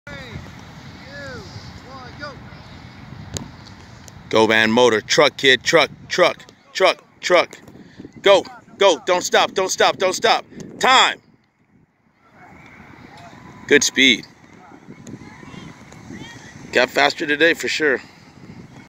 SMBA Sports Performance Training - Running the 60 - Personal Best!
At SMBA camp, players learn proper running techniques and then use Resistance Training with parachutes all week to build up their performance time! On Friday, they see if they got faster!